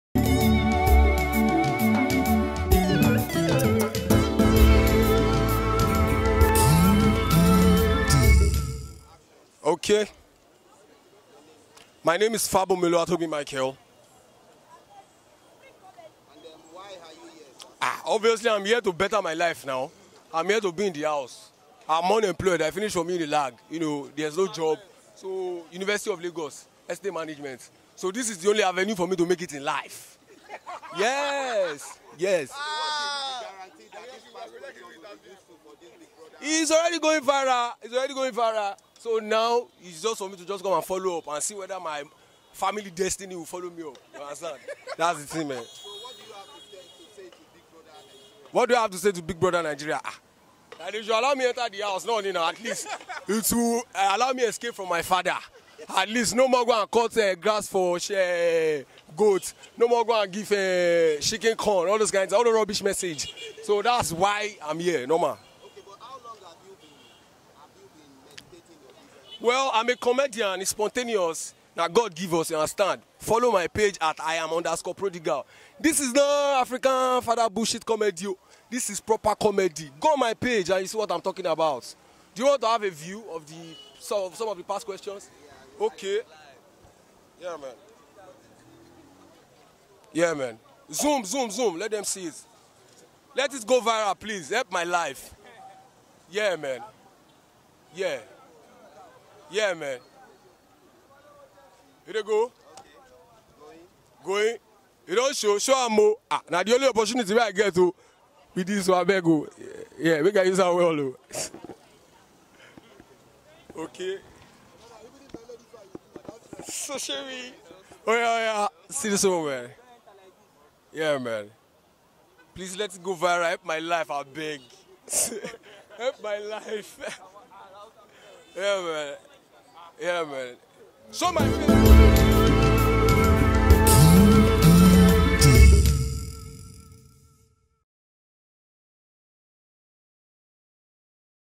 Man selling Big Brother Naija past questions | Funny flashback video
QedTV was at the venue of the audition for the 2019 Big Brother Naija reality TV show and met with a man selling past questions.